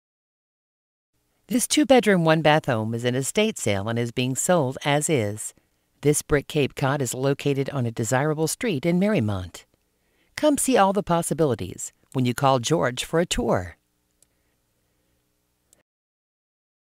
6989 Cambridge Ave Mariemont, OH Homes for Sale
6989 Cambridge Ave, Mariemont, OH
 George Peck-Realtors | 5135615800

Estate Sale: Sold As Is 2 brm, 1 bath brick cape cod on desirable street in Mariemont